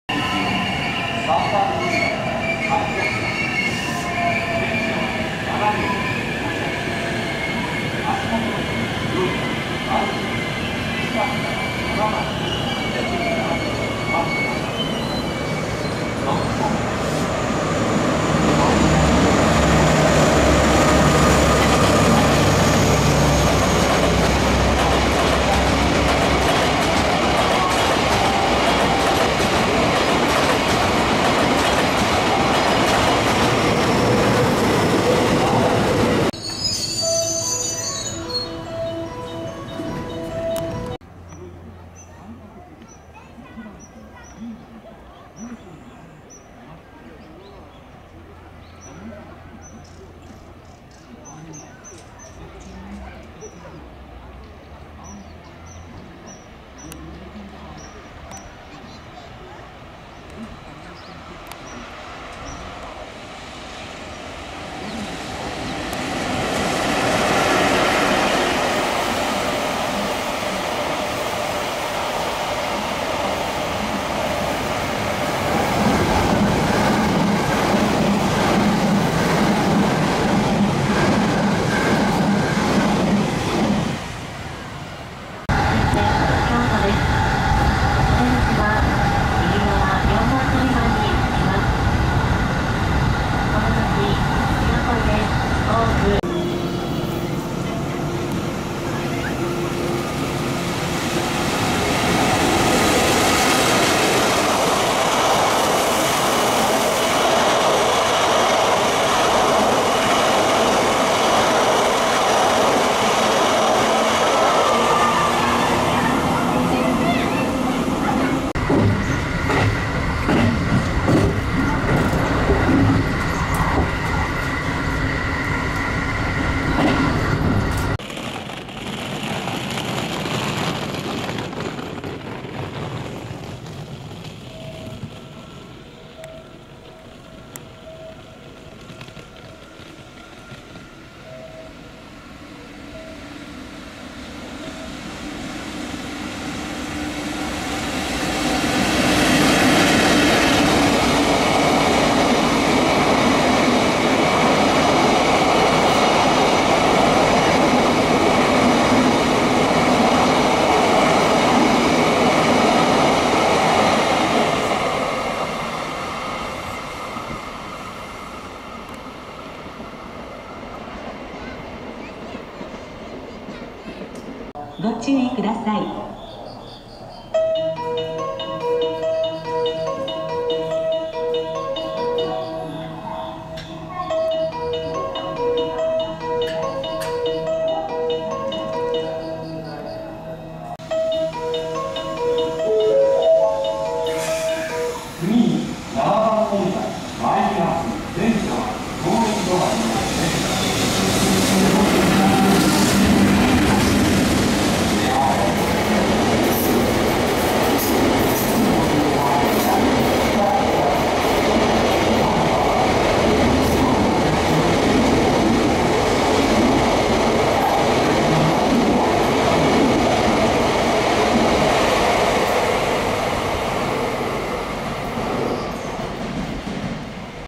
[4K]小田急5000形電車甲種輸送(20191101) Delivering Odakyu 5000 EMU
小田急5000形電車甲種輸送(20191101) Delivering Odakyu 5000 EMU
東淀川/山崎/京都貨物/山科/能登川/米原
Shot at HigashiYodogawa,Yamazaki,Kyoto Freight Stn.,Yamashina,Notogawa,and Maibara.